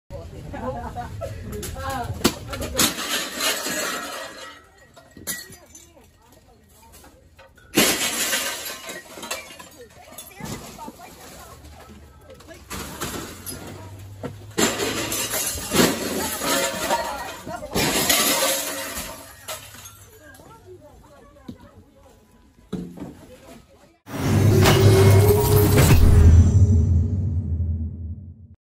More looting 'protesters' in Philadelphia
PHL - More looting was reported across Philadelphia late Wednesday and early Thursday despite a citywide curfew in the wake of the deadly police shooting of Walter Wallace Jr., CBS Philly reports. More than half the stores in a northeast Philadelphia strip mall were vandalized overnight.

One of the stores' managers told the station a group of about 12 to 15 people drove up to the shopping center at about midnight and went business by business, smashing doors and windows.

"We were inside, and we were doing a couple of cleaning jobs and everything, trying to spruce the place up at the time and we saw a bunch of cars pull up so we hit the lights," Redz Bar and Grill manager Kevin Donahue said. " … They came up with bats and just start smashing windows.